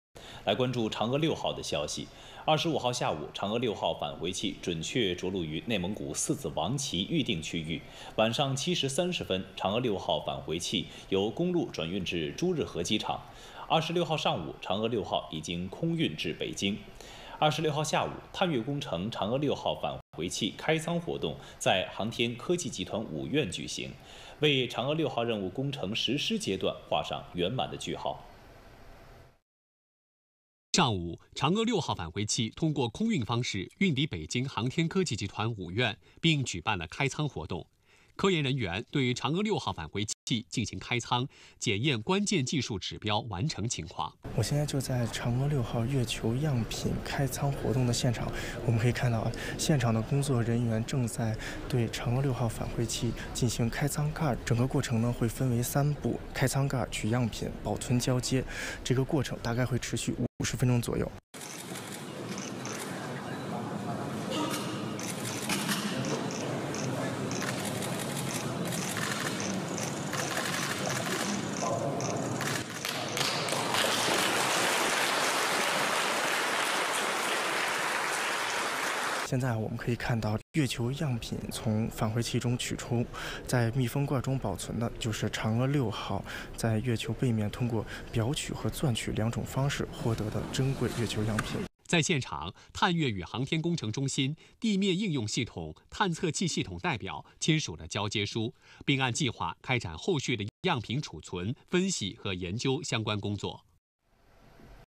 嫦娥六号返回器完成开舱取样
6月26日下午，探月工程嫦娥六号返回器举行开舱活动。后续，地面应用系统的科研人员将按计划开展样品储存、分析和研究相关工作。嫦娥六号探测器于5月3日发射升空，历经53天，首次获取月背的月球样品，于6月25日与轨道器分离并携带月背样品重返地球。（视频来源：中央广播电视总台）